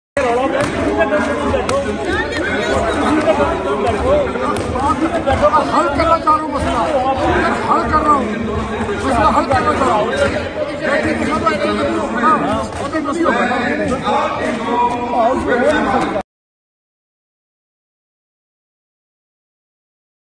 June 16, 2021
Rana SanaUllah Khan's giving the shut up call to Shah Mahmood in Assembly session